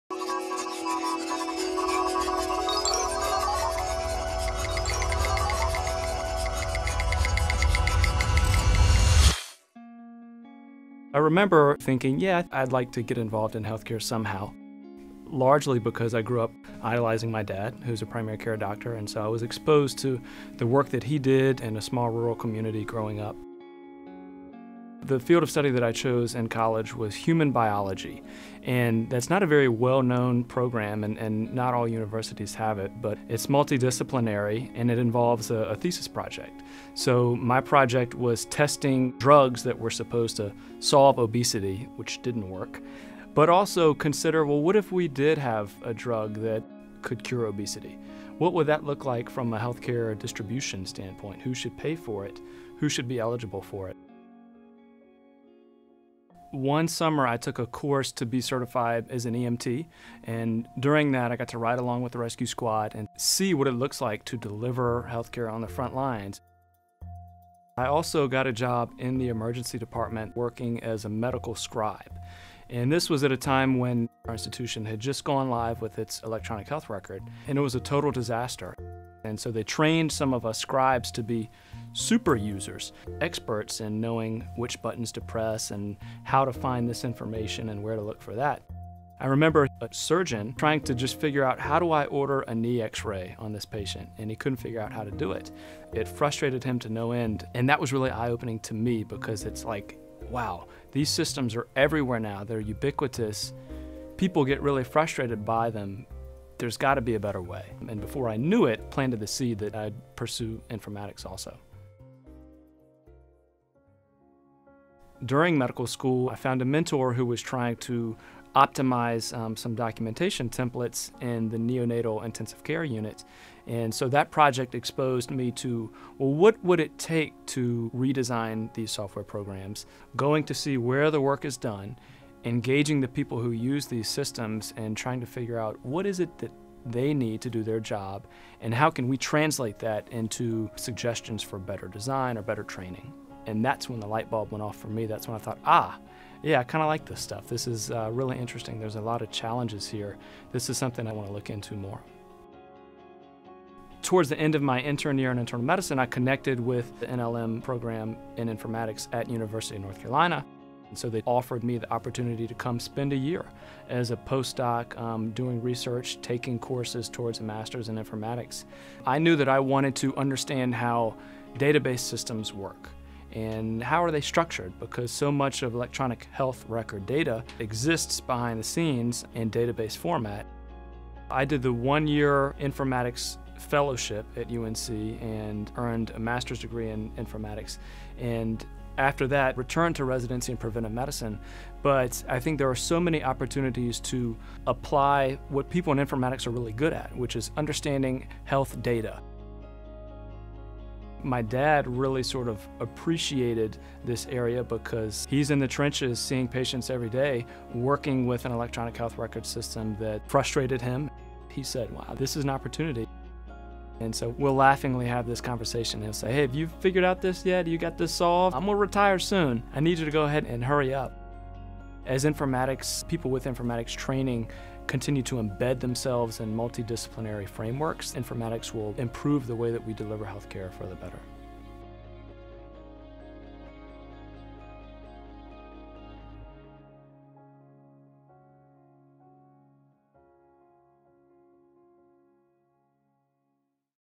Connections: Cameron Coleman, Jr., MD, MPS | Career Paths in Biomedical Informatics
Cameron Coleman, Jr., MD, MPS

Cameron Coleman is completing his residency in preventive medicine at the University of North Carolina Hospitals. He received a master’s degree in biomedical and health informatics, which he is using to focus on quality improvement and population health.

Always on a Pre-Med Path

Cameron Coleman grew up in a small, rural community in Virginia. After concluding that his childhood desire to drive an ice cream truck was probably not a sustainable career, he set his sights on healthcare. He had significant exposure to healthcare, as his father was a primary care doctor.

Cameron liked most academic subjects, particularly math and science. In entering college at the University of Virgina, Cameron decided to pursue a pre-med track, taking courses in biology and chemisty, and majoring in human biology. His thesis involved studying supposed miracle treatments for obesity, which he concluded didn’t work.

One summer during college Cameron became certified as an EMT. He rode with a rescue squad and saw care delivered on the front line, which was thrilling and reinforced his interest in healthcare. He also worked as a medical scribe at a healthcare facility. This institution had just implemented an electronic health record (EHR), which was a disaster. The institution had to bring in pre-med students to help clinicians with documentation. This experience exposed Cameron to the complexity of the healthcare system and the importance of technology, systems, and processes. Cameron believes this experience planted the seed for his interest in informatics.

Cameron took a year off after college to work as a full-time scribe in an emergency department. While this was a different healthcare facility than he worked at previously, he saw the same frustrations in adopting EHRs and saw the same need to optimize IT systems and improve processes.

Solving Big Problems

Cameron continued on to medical school at the University of Virginia. In med school he got involved in a research project focused on optimizing EHRs and making them easier to use. This was his first exposure to informatics.

During his internship at the University of North Carolina Hospitals, Cameron was frustrated with the IT systems being used and the negative impact these systems had. His frustration caused him to reflect on how he might use his medical training to solve bigger, systemic problems.

At the end of his UNC internship, Cameron had an opportunity to spend a postdoc year doing research and taking courses in informatics, which resulted in pursuing a Master’s in Biomedical and Health Informatics. Because he didn’t have a technology or programming background, Cameron initially felt a bit overwhelmed. He focused on learning about computer programming, databases, and how EHRs work. Over time he grew comfortable with the language and tools of biomedical informatics.

Cameron was excited about being able to do translational informatics in the real world, work on hospital quality improvement initiatives, and use skills he acquired, such as querying database systems to identify patients.

New Career Paths

While at UNC, the director of the National Library of Medicine spoke at the institution. She said in the past, the pathway to a healthcare career was through clinical work. In the future, a career in healthcare will be through technical work. This perspective opened Cameron’s eyes to possibilities gained through his skills and experiences. Cameron says his degree in biomedical informatics has “opened a lot of doors.”

As Cameron completes his residency in preventive medicine, he sees patients a few days each week, but is also thinking more broadly. He is focused on improving population health and working on hospital and outpatient quality improvement initiatives, and spends time with state and local health departments.

Cameron sees technology and informatics becoming more integrated with public and population health. As technology becomes better integrated, he believes it will become more intuitive and easier to use. It will make it easier and more fun for physicians like his father to practice medicine, which will ultimately improve patient care and population health.

The video project is funded by the National Library of Medicine Administrative Supplement Funds to the Harvard Medical School Biomedical Informatics and Data Science Training program, 3T15LM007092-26S1.